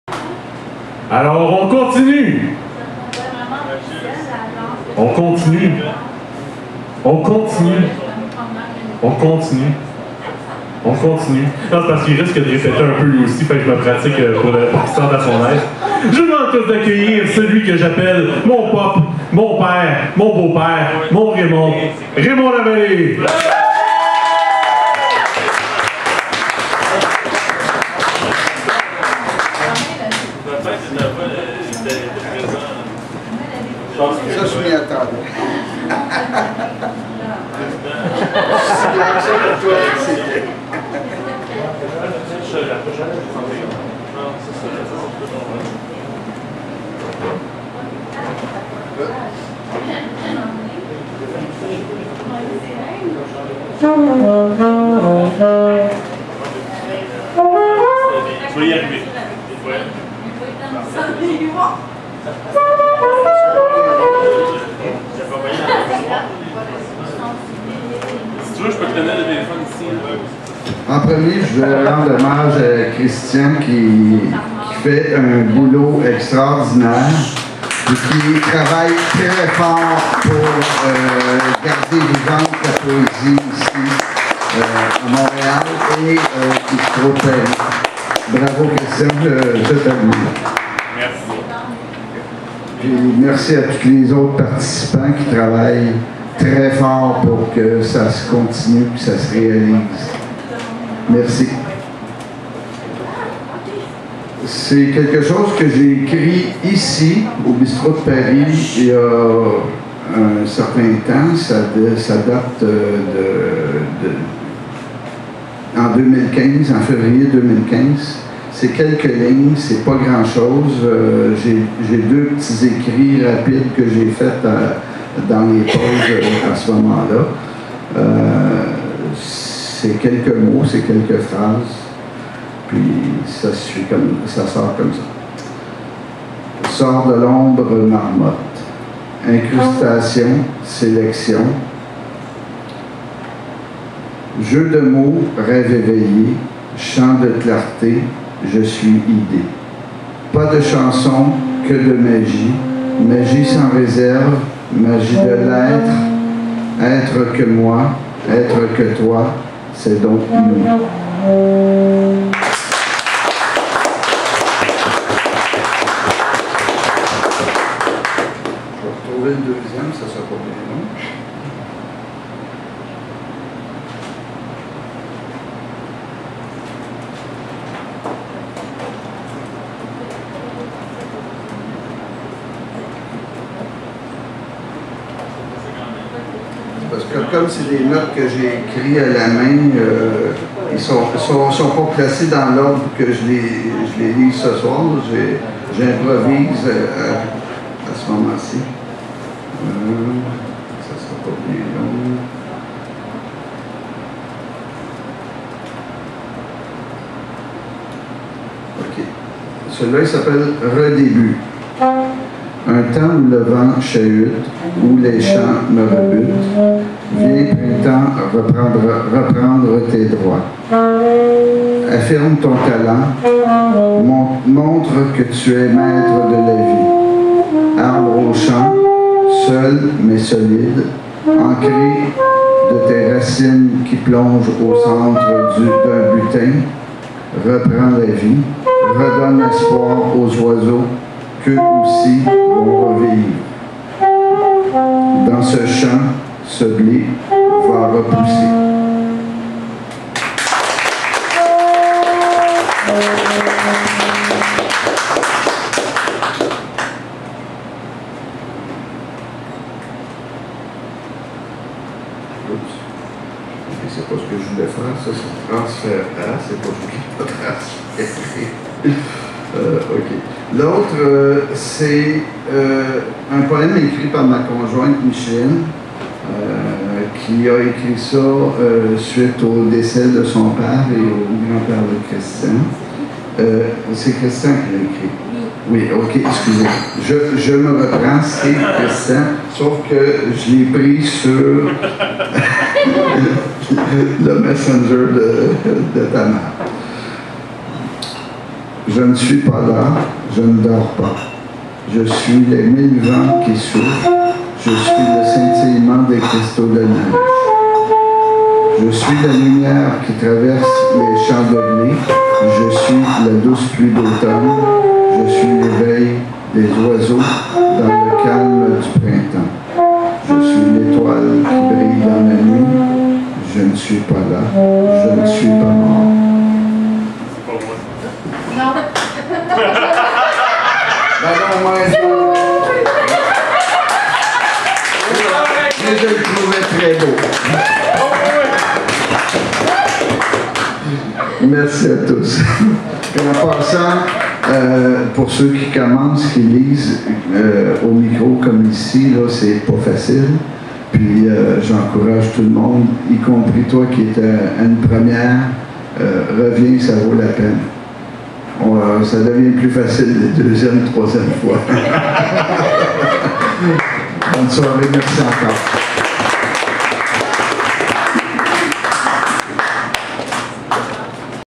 Raymond Lavallée au retour de La Messe Poétique 02
Raymond Lavallée

Le retour de La Messe Poétique 02 au Bistro de Paris le lundi le 2 juillet dès 18h00 au Bistro De Paris Bistro. Nous recevrons Mylène Lavoie Conteuse Poétesse en tant qu'artiste invitée. En musique au saxophone avec Connor Leclair. Le tout sera documenté sous vidéos par Loup Solitaire.